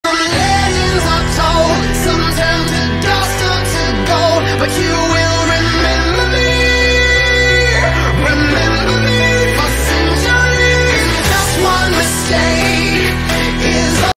Why God Really Created A Pillar Of Fire For The Israelites 🔥🔥
Exodus 13:21
"The Lord preceded them, in the daytime by means of a column of cloud to show them the way, and at night by means of a column of fire to give them light. Thus they could travel both day and night."